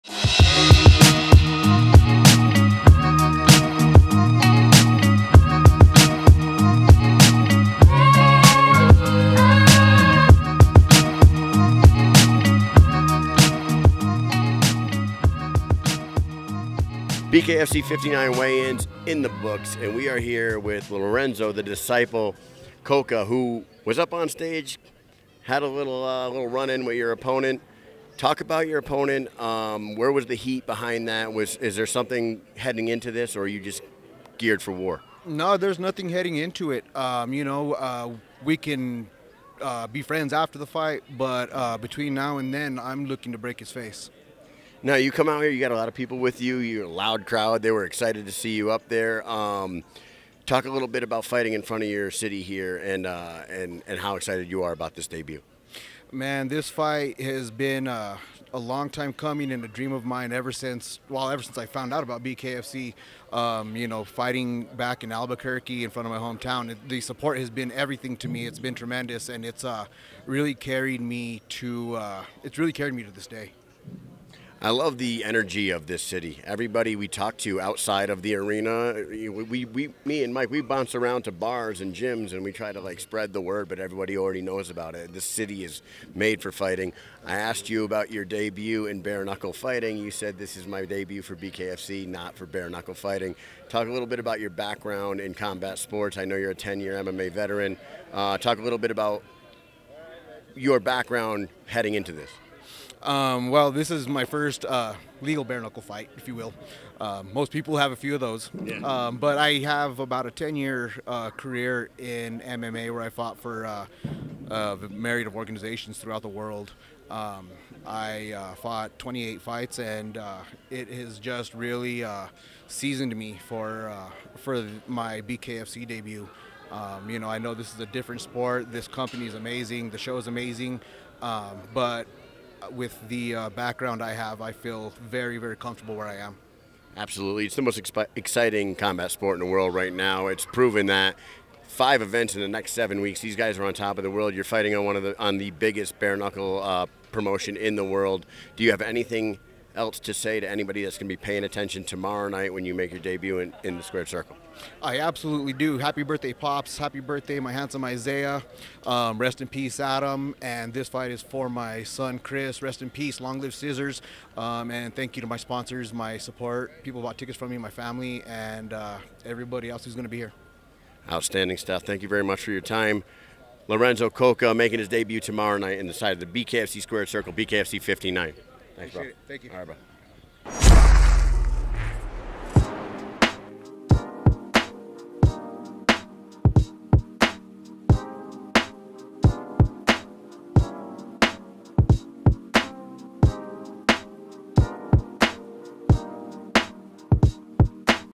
BKFC 59 Weigh Ins Interview with Lorenzo Coca "I'm looking to break his face"
Lorenzo "The Disciple" Coca is an MMA veteran that is making his bare knuckle fighting debut at BKFC 59 when he faces off against Ruben "Bad Boy" Warr. There was some heat between the two combatants at the weigh ins and we're looking forward to a war.

Bucked Up supplements and energy

Dynamic Life
Weight-loss, sexual health, and anti-aging
Become a patient in just 20 seconds!